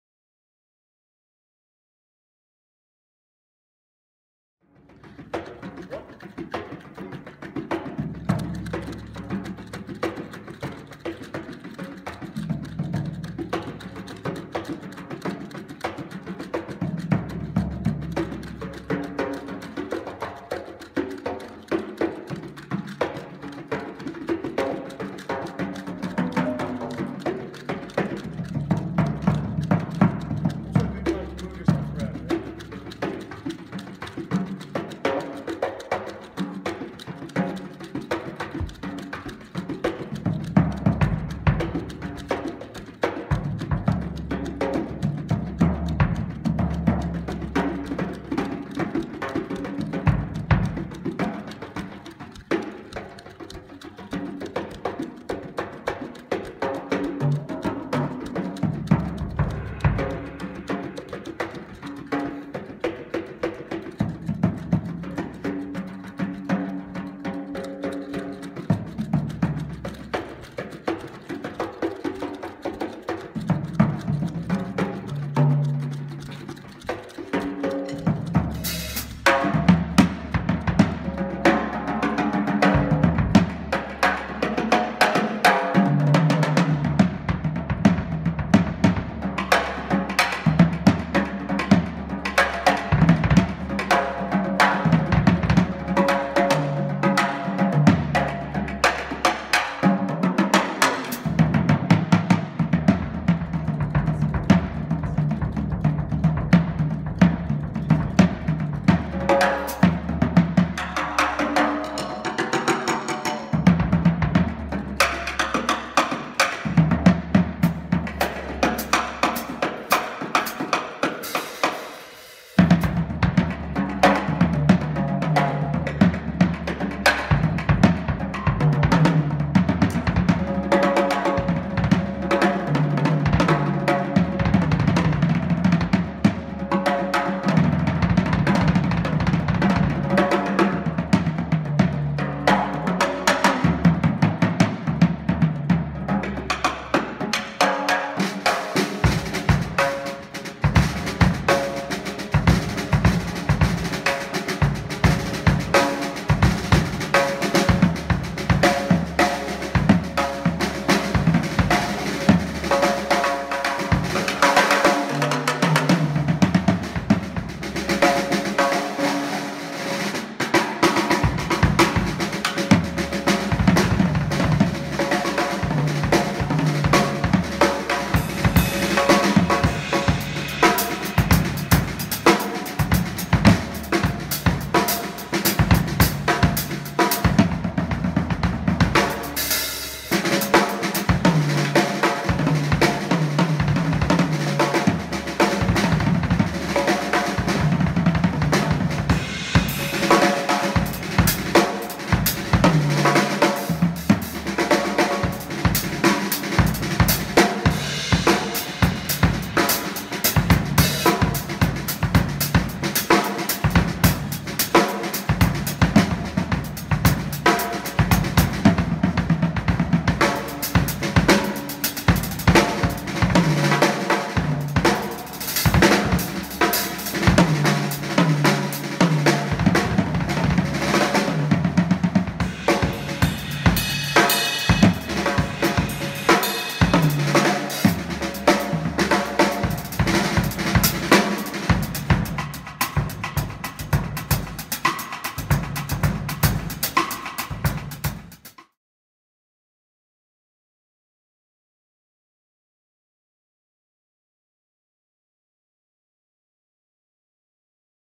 Groove Regulators Edu Clinic 08 December 2018
Groove Regulators hosted a drumming workshop for some young drummers on Saturday 08 December 2018.